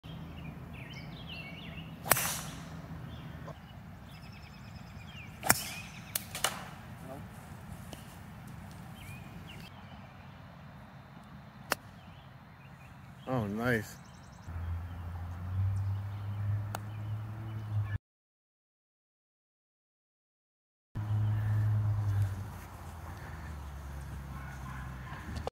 Par 4 in Parma Ohio
Tom and I are at Ridgewood Golf Course in North East Ohio. This is a Par 4 hole and it is just one of the holes that we decided to record at in this video.

Tags Shorts short

prior service vet.   U.S. Army retired 74d CBRNE